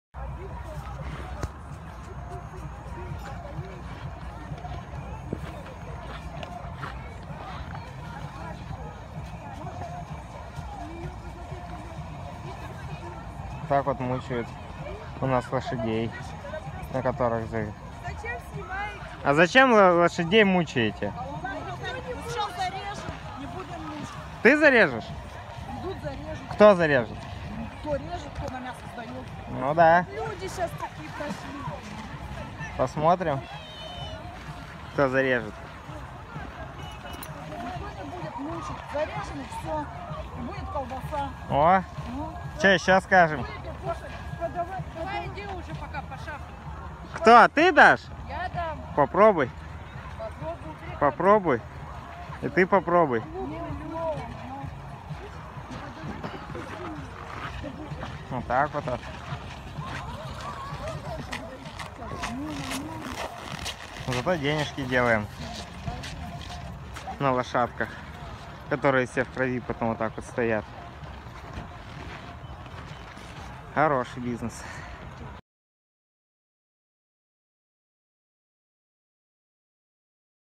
Уставшая лошадь
Инцидент с падающей лошадью на острове Татышев в Красноярске